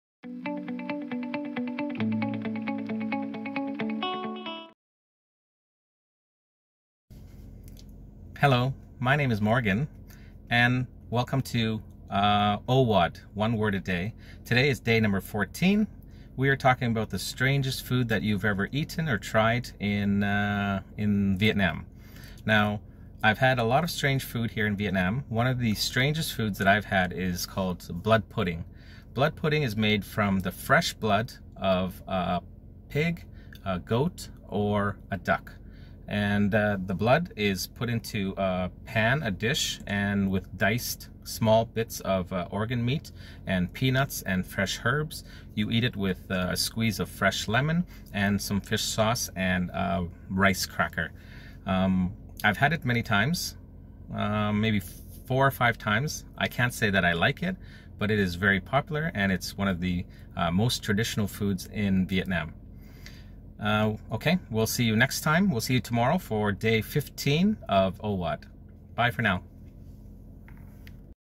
OWAD Day 14: Strange foods in Vietnam - blood pudding
What's the strangest food you've ever eaten?